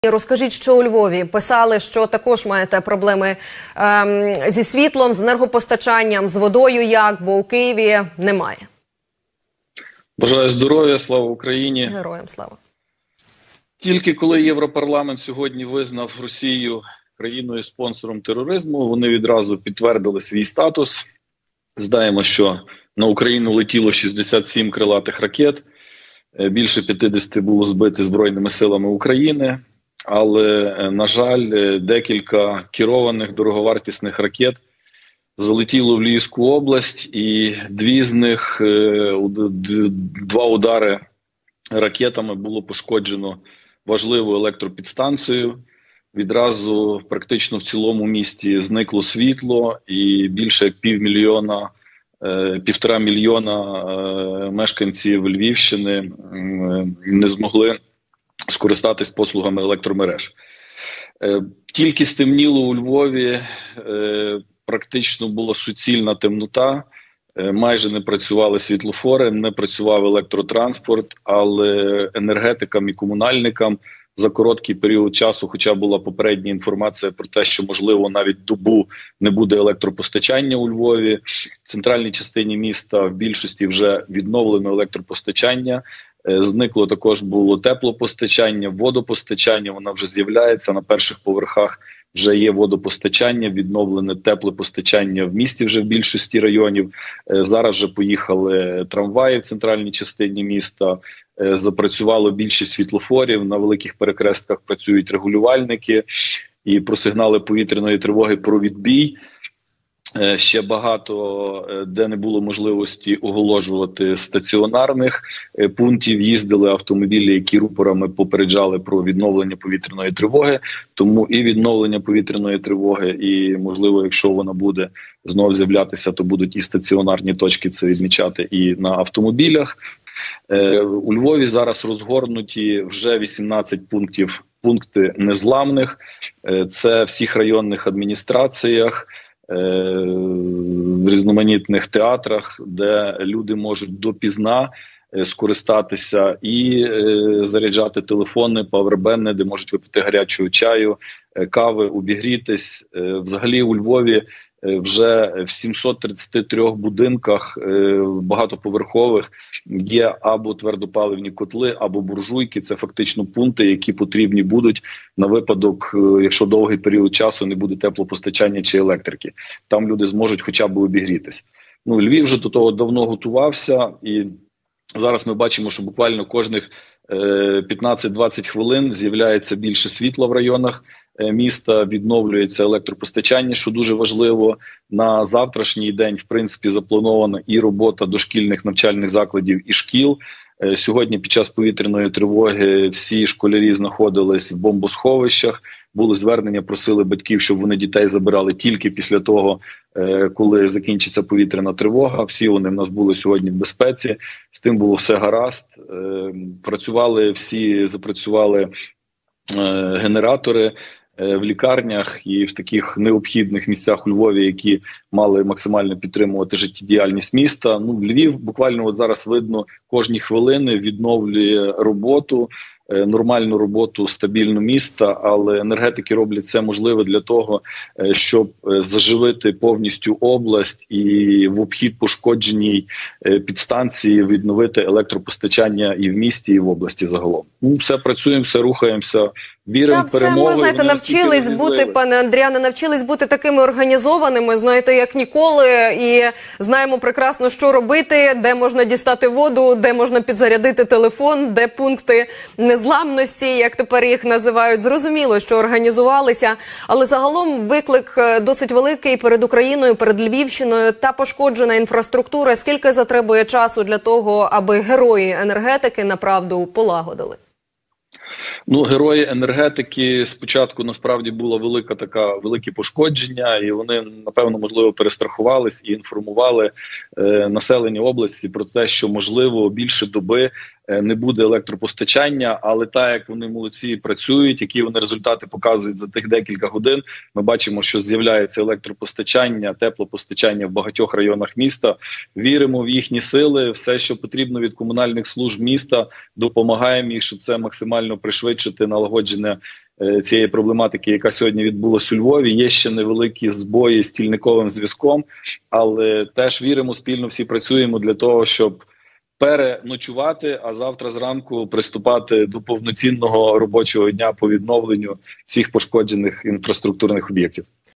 Львів: комунальники роблять все можливе для швидкого налагодження всіх систем, — Андріян Гутник
Російський ракетний удар по Україні 23 листопада: про ситуацію у Львові інформує депутат міської ради, свободівець Андріян Гутник. Етер телеканалу "Апостроф ТБ" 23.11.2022 року.